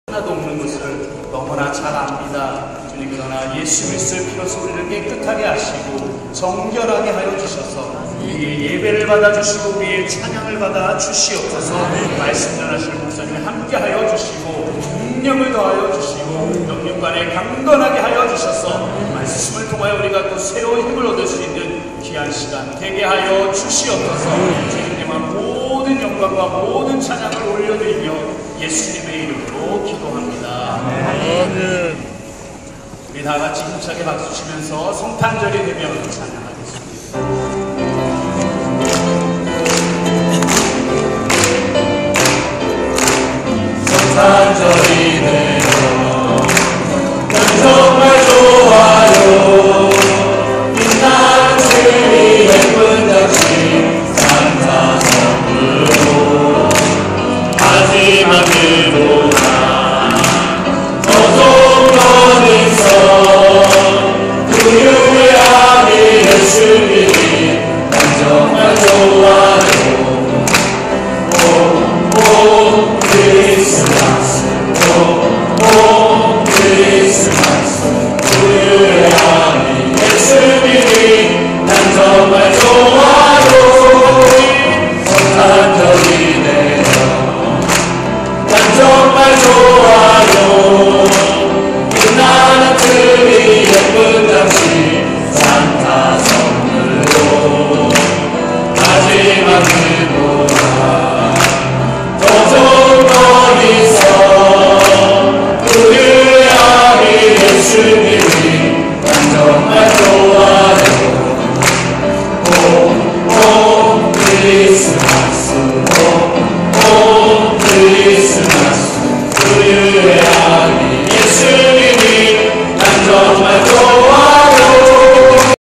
성탄 이브 수요기도회시 준비찬양
명성교회 성탄 이브 수요 기도회시 준비찬양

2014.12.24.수

이명철 목사님 (영어예배 담당)